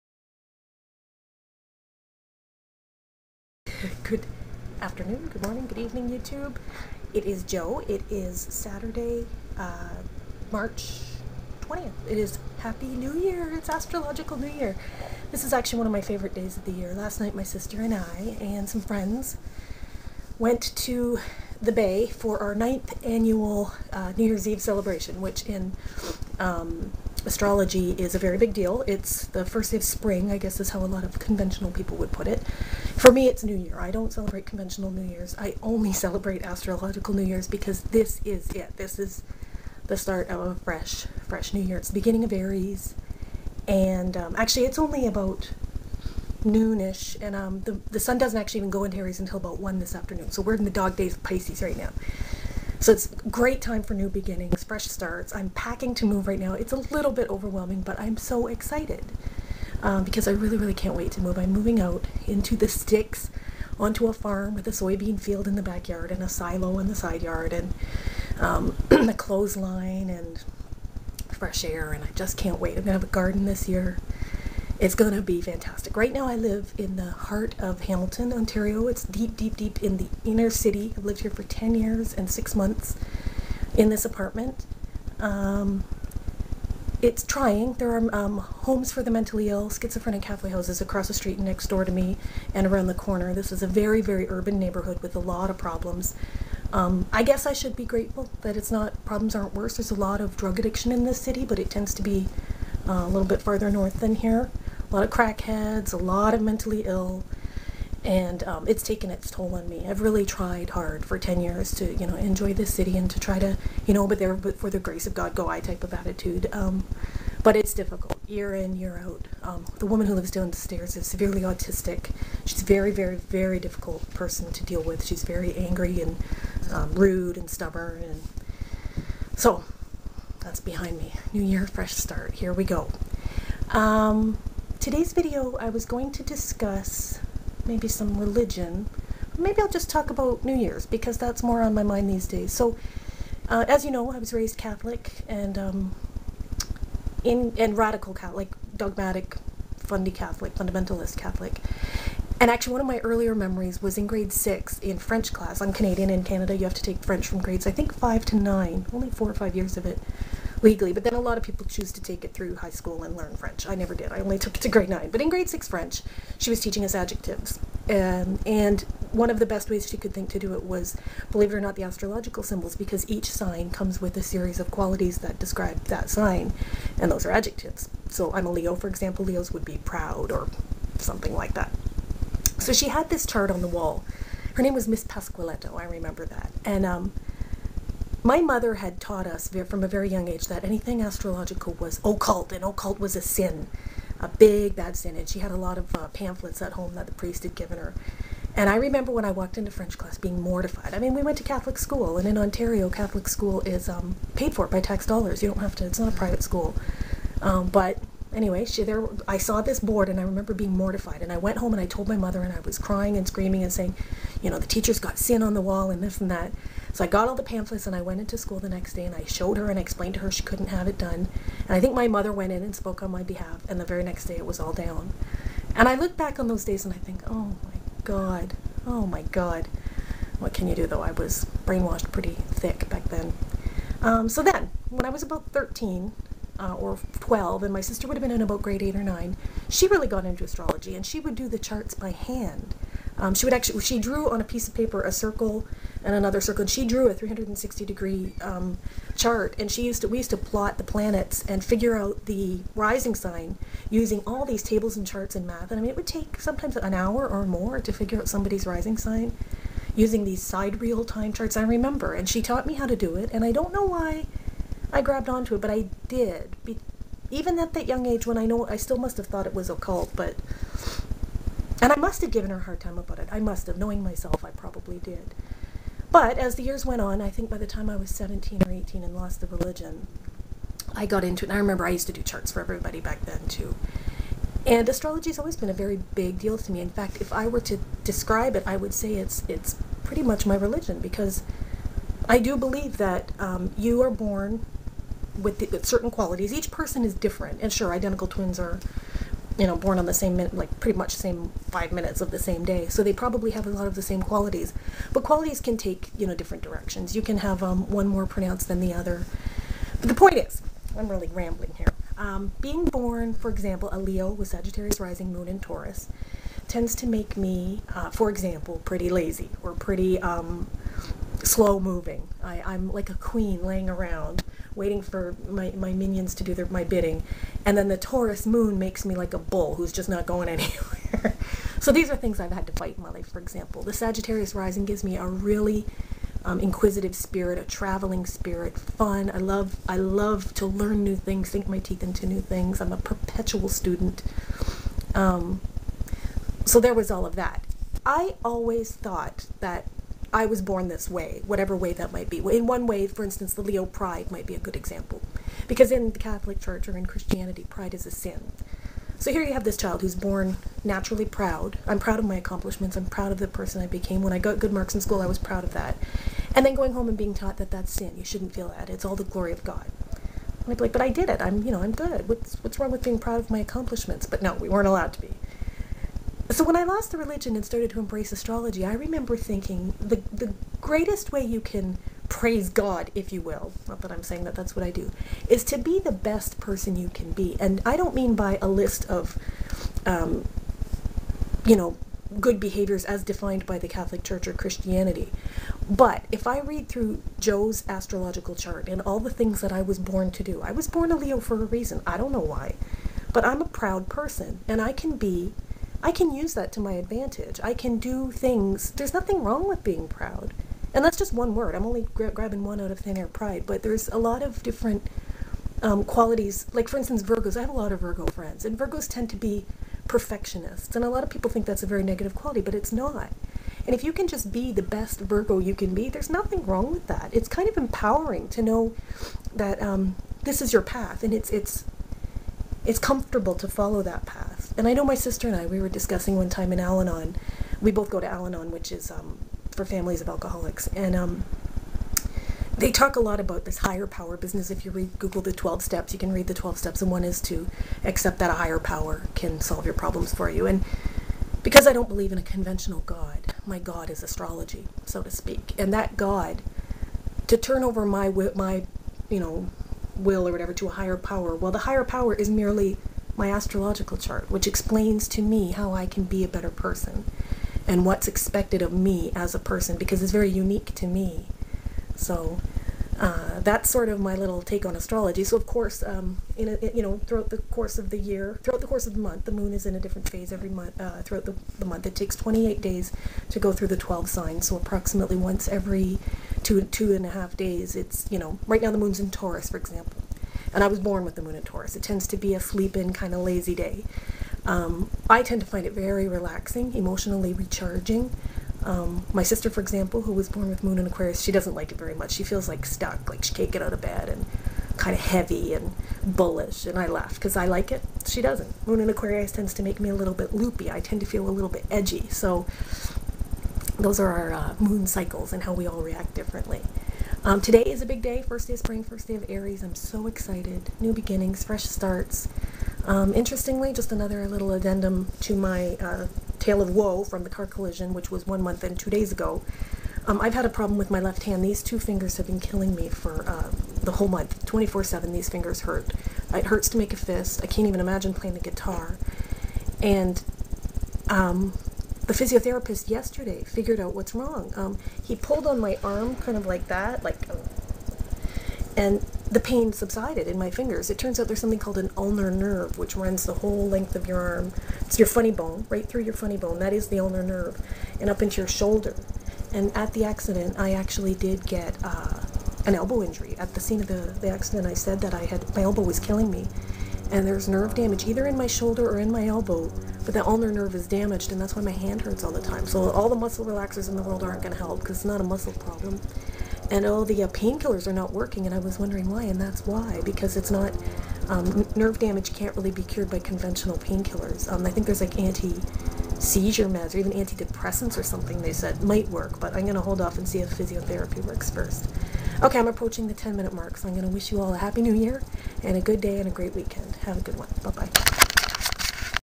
Happy New Year!
Today is astrological new years, otherwise known as the first day of spring, 2010.  This is one of my favourite days of the year.  In this video, I discuss how I came to believe that astrology is my personal 'higher power' and how I can deduce what my path is based on using my astrological chart as a guide.